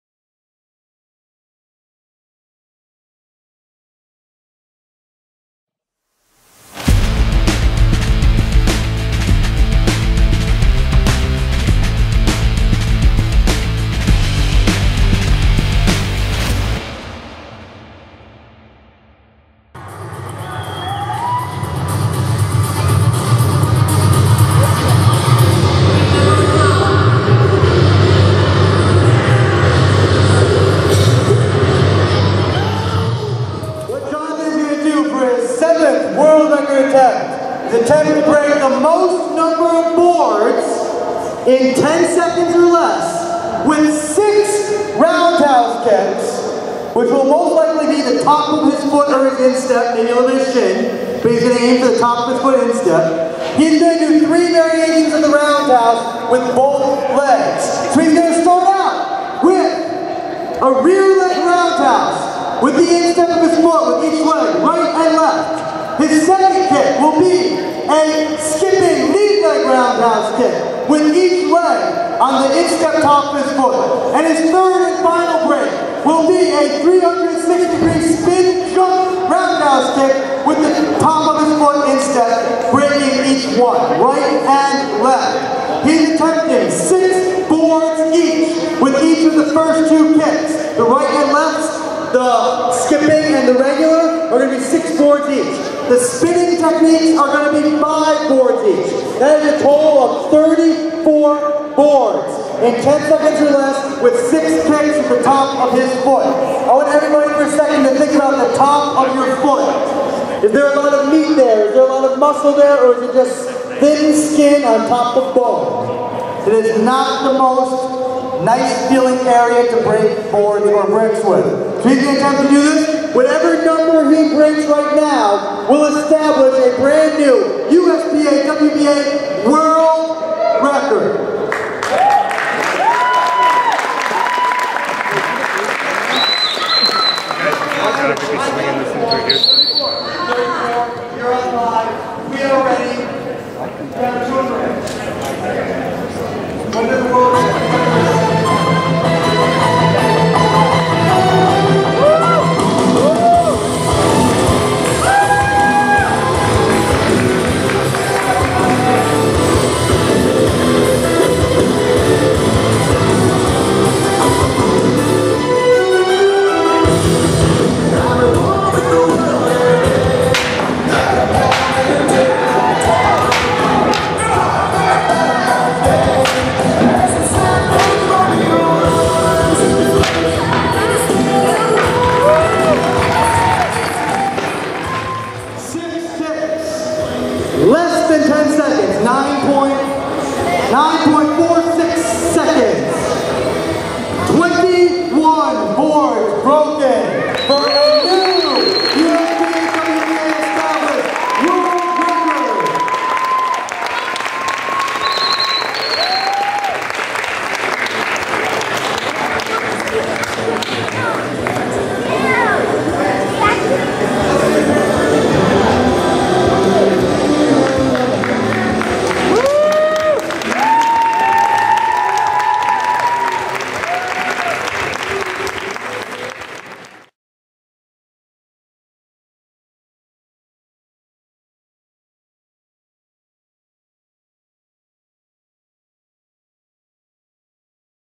USBA/WBA World Record- Jonathan Field- 21 boards with 6 Roundhouse Kicks in 10 sec- 8/2018
New World Record set by Jonathan Field of Cobourg, Ontario, Canada, in Albany, NY, USA. 21 one inch boards broken with 6 Roundhouse Kicks (rear leg, lead leg, 360 jumping roundhouse) in 10 seconds.